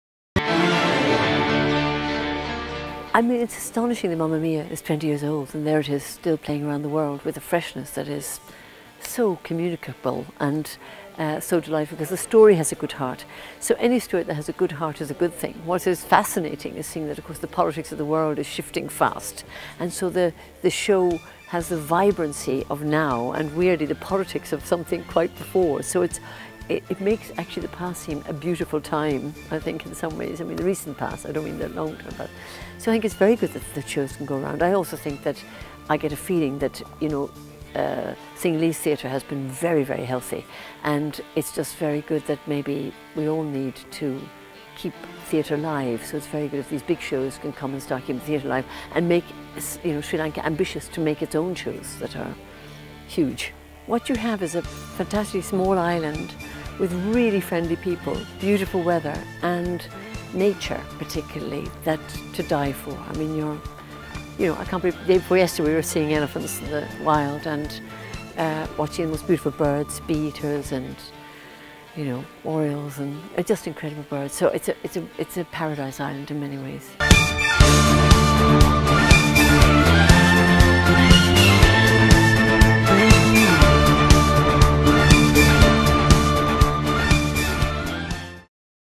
Fiona Shaw I MAMMA MIA!
Acclaimed British actress Fiona Shaw sharing her thoughts on the show.